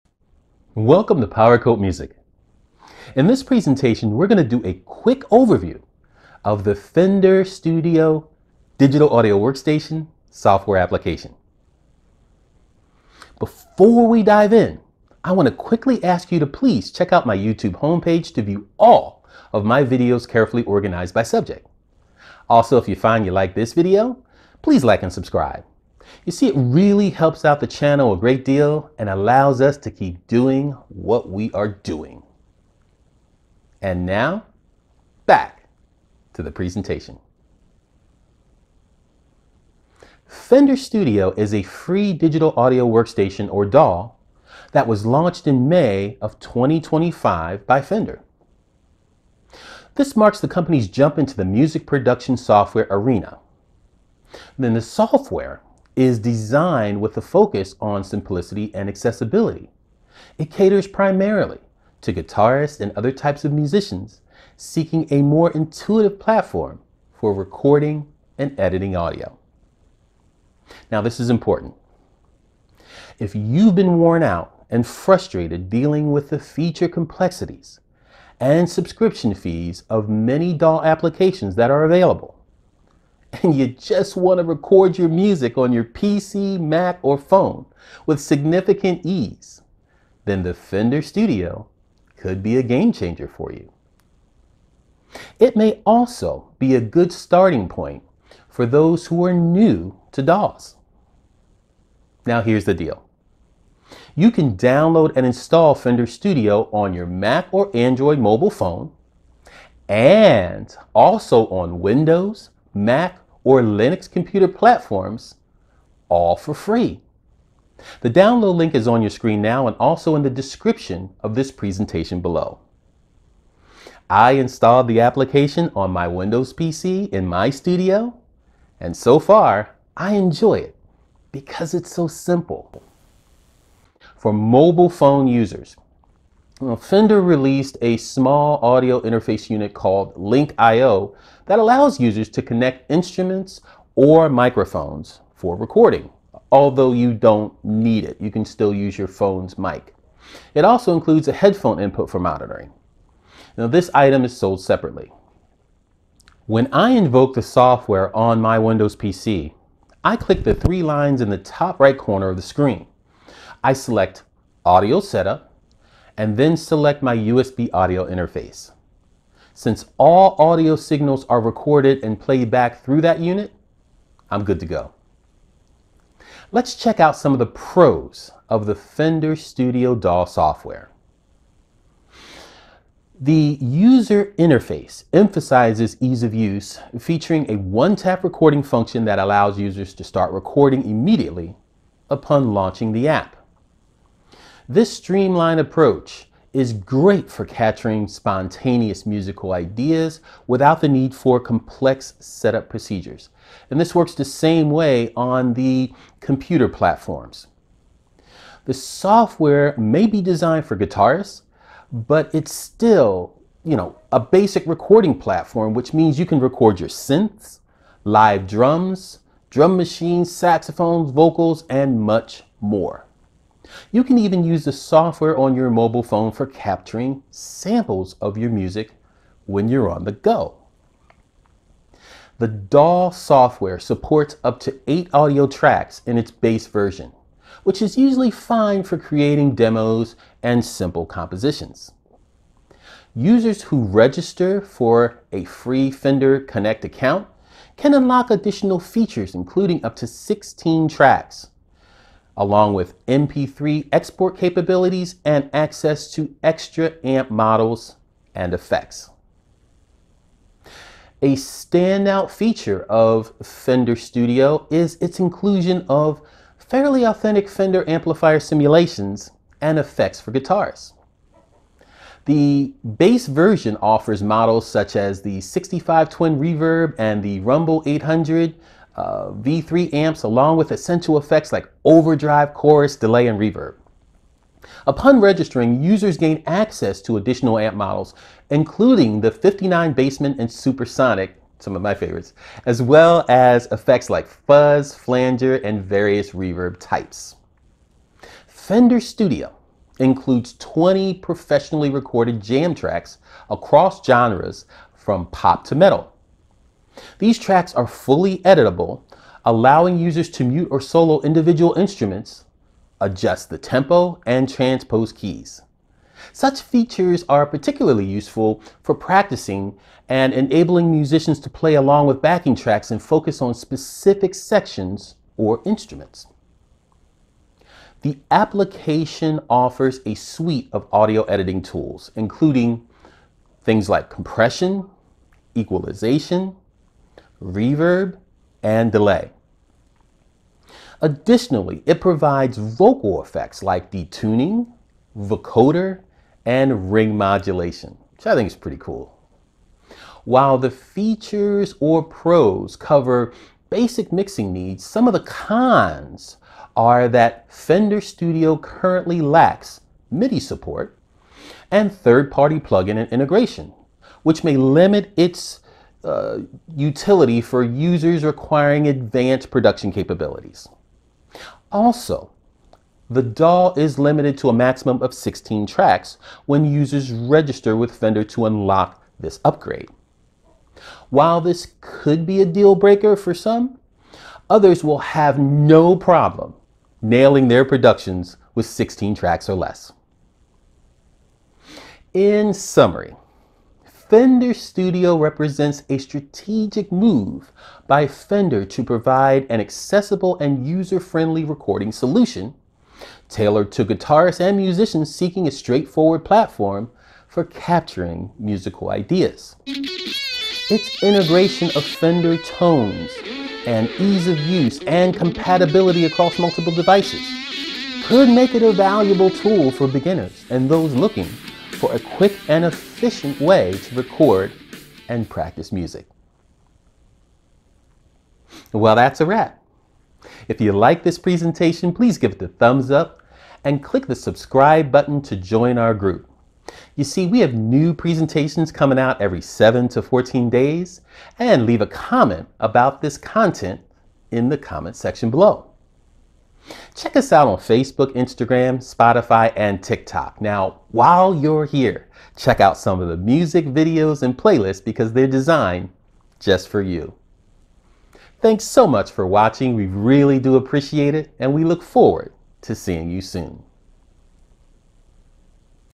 Fender Studio: FREE Mobile & PC DAW Quick Overview
Fender Studio is a FREE DAW software application launched in May 2025 by Fender, marking the company's jump into the music production software arena. The software is designed with a focus on simplicity and accessibility, and caters primarily to guitarists and other types of musicians seeking a more intuitive platform for recording and editing audio. In this presentation, I do a quick “No Nonsense” overview of the application with PROS and CONS to give you a good idea of what it can do, and who may work best for. Watch all of this presentation now to get the information you want and deserve.